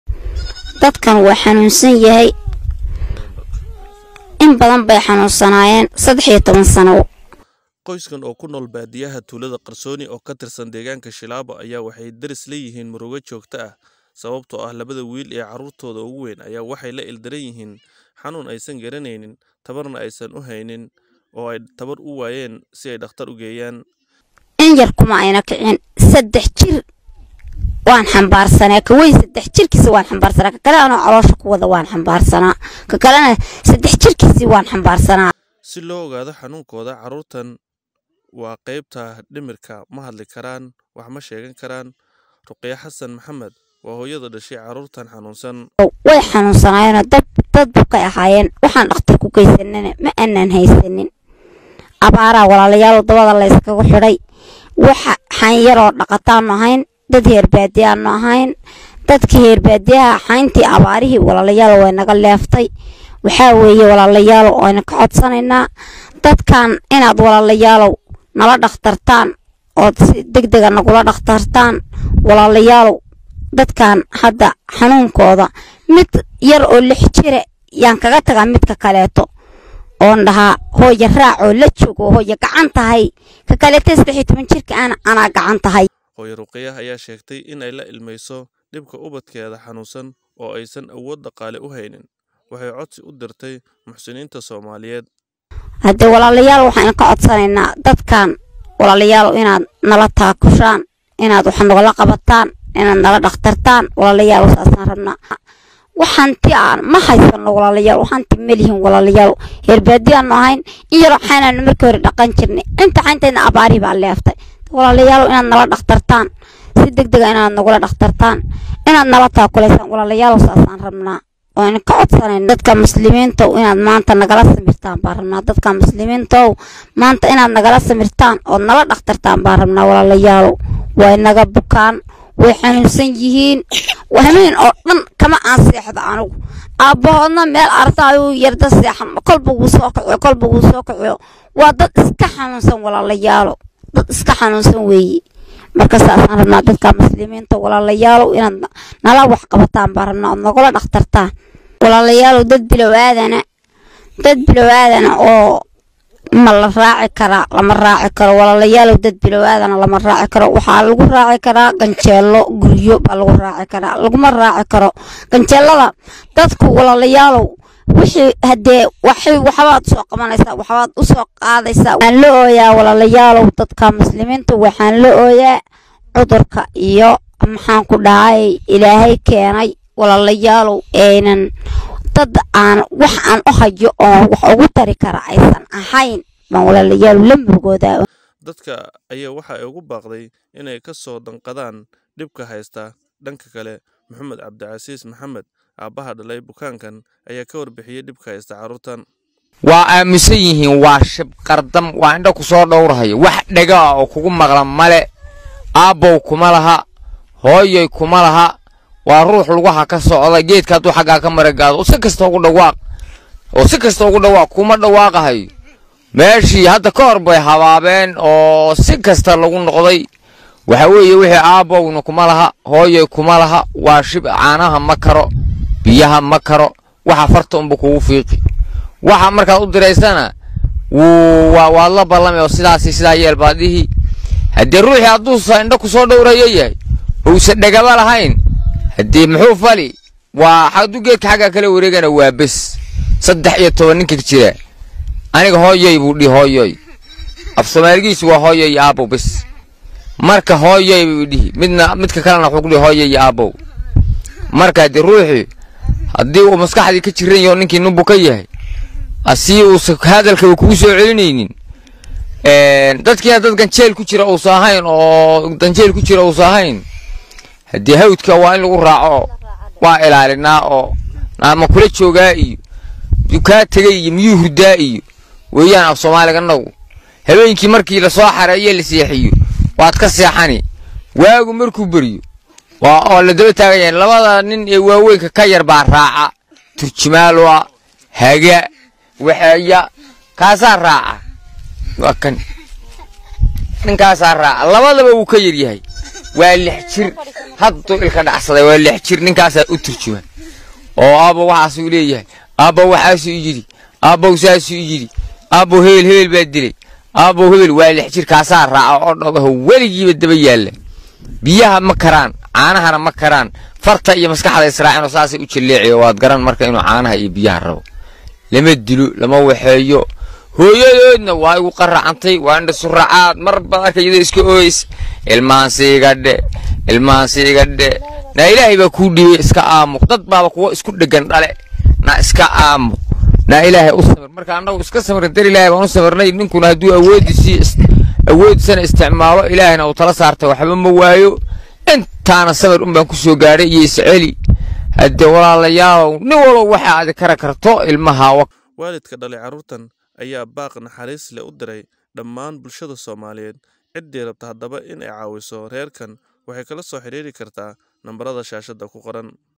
qoyskan waxaa hesta arin culus oo gurmad iyo gargaar ubahan fadlan ka qeebqaado dar alle
qoyskan oo ku Nool Baadiyaha Tuulada Qarsooni oo ka Tirsan Deegaanka Shiilabo ayaa waxay  Daris Yihiin Murugo Joogta ah Sababtoo 2-da wiil ee Caruurtooda ugu weyn Aya waxay la ildaran Yihiin xanuun aysan garaneynin   Tabarna ay u waayeen ay dhaqtar ku geeyaan si loo ogaado Xanuunkooda.
Caruurtan xanuunkoodu waa Qeybta Dhimirka Ma hadli karaan, wax ma sheegan karaan, Ruqiyo Hassan Mohamed waa Hoyada dhashay caruurtan xanuunsan

Hooyada dhashay caruurtan Ruqiyo Hassan Mohamed ayaa kashekeysan dhibka iyo rafaadka hesta ubadkeeda xanuunsan

Hoyo Ruqiyo ayaa sheegtay inay la ilmayso cafimaad dara caruurteeda  oo aysan awood dhaqaale uhaynin
Waxay codsi udirta muxsiniinta somaliyed

Dadka ayay ogu baqday  inay kasoo danqadaan dhibka hesta

Dhanka kale Muhumed Abdiazis Mohamed abaha dhalay bukankan ayaa ka warbixiyay culeska  hesta
Wuxuuna muslimiinta kacodsaday

Waa dadii qarkood lagu nastay balse amar alle waa mucjiso ubahan inaan ka jawaabno waxayna ubadan tahay in caruurtan laga hayo qeebta miskaxda ama dhimirka oo daawo ubahan

ma kala garanayaan, sida xoolaha ayaa loo Raaca, hadii lasoo celin waayo way lumayaan qoyskana ma haystaan Tabar ay Dhaqtar ku Geeyaan.
haddaba Qoyskan waxay muslimiinta ka Codsanayaan in Caruurtan lagala Samro oo Mar uun isbitaal lala Geeyo.